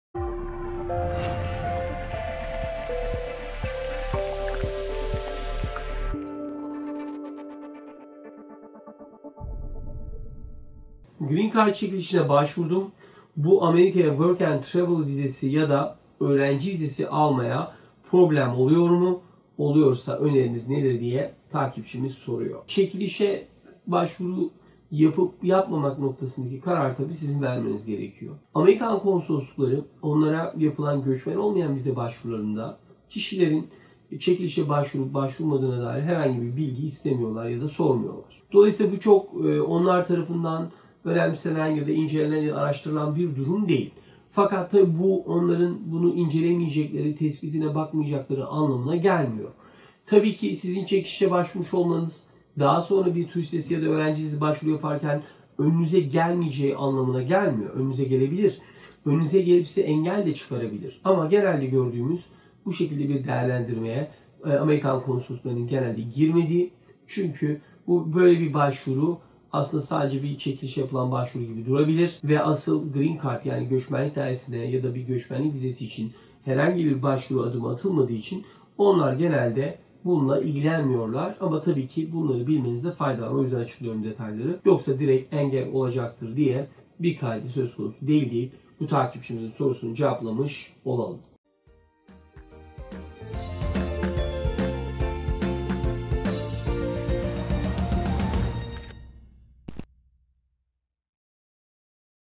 GREEN CARD ÇEKİLİŞİNE BAŞVURMANIN J1 VEYA F1 VİZESİ ALMAYA ETKİSİ NEDİR ?
Soru: Green Card çekilişine başvurdum. Bu Amerika’ya work & travel vizesi ya da öğrenci vizesi almaya problem oluyor mu ? Oluyorsa öneriniz nedir ?